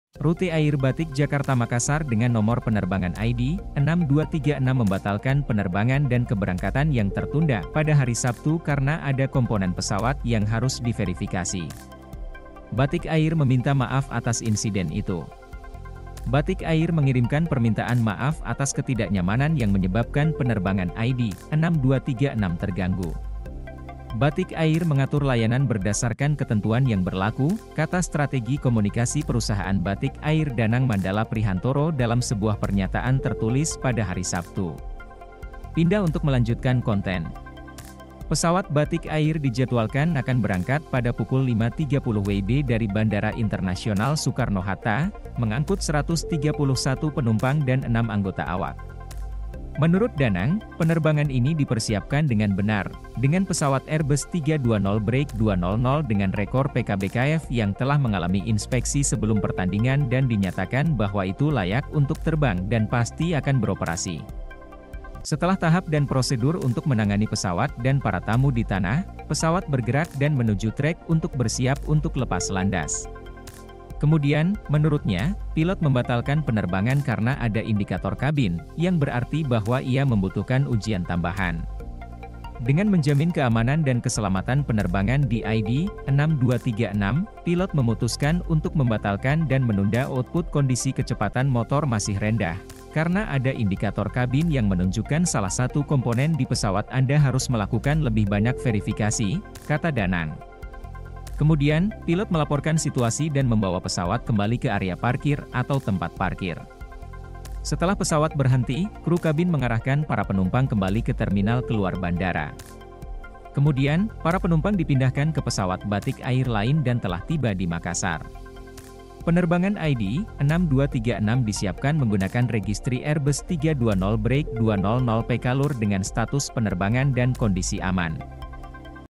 Kronologi udara Batik gagal lepas landas di bandara Soetta pada Sabtu pagi
Pesawat Batik Jakarta-Makassar dengan nomor penerbangan ID-6236 membatalkan penerbangan (lepas landas) dan ditunda keberangkatan pada hari Sabtu (30/7).

Rute Air Batik Jakarta-Makassar dengan nomor penerbangan ID-6236 membatalkan penerbangan dan keberangkatan yang tertunda pada hari Sabtu karena ada komponen pesawat yang harus diverifikasi

Batik Air meminta maaf atas insiden itu. "Batik Air mengirimkan permintaan maaf atas ketidaknyamanan yang menyebabkan penerbangan ID-6236 terganggu. Batik Air mengatur layanan berdasarkan ketentuan yang berlaku," kata strategi komunikasi perusahaan Batik Air Danang Mandala Prihantoro dalam sebuah pernyataan tertulis pada hari Sabtu. Pindah untuk melanjutkan konten

Pesawat Batik Air dijadwalkan akan berangkat pada pukul 05.30 WIB dari Bandara Internasional Soekarno-Hatta, mengangkut 131 penumpang dan enam anggota awak. Menurut Danang, penerbangan ini dipersiapkan dengan benar, dengan pesawat Airbus 320-200 dengan rekor PK-BKF yang telah mengalami inspeksi sebelum pertandingan dan dinyatakan bahwa itu layak untuk terbang dan pasti akan beroperasi. Setelah tahap dan prosedur untuk menangani pesawat dan para tamu di tanah, pesawat bergerak dan menuju trek untuk bersiap untuk lepas landas. Kemudian, menurutnya, pilot membatalkan penerbangan karena ada indikator kabin, yang berarti bahwa ia membutuhkan ujian tambahan. "Dengan menjamin keamanan dan keselamatan penerbangan di ID-6236, pilot memutuskan untuk membatalkan dan menunda output kondisi kecepatan motor masih rendah, karena ada indikator kabin yang menunjukkan salah satu komponen di pesawat Anda harus melakukan lebih banyak verifikasi, "kata Danang. Kemudian, pilot melaporkan situasi dan membawa pesawat kembali ke area parkir atau tempat parkir. Setelah pesawat berhenti, kru kabin mengarahkan para penumpang kembali ke terminal keluar bandara. Kemudian, para penumpang dipindahkan ke pesawat Batik Air lain dan telah tiba di Makassar. "Penerbangan ID-6236 disiapkan menggunakan registri Airbus 320-200 PK-Lur dengan status penerbangan dan kondisi aman.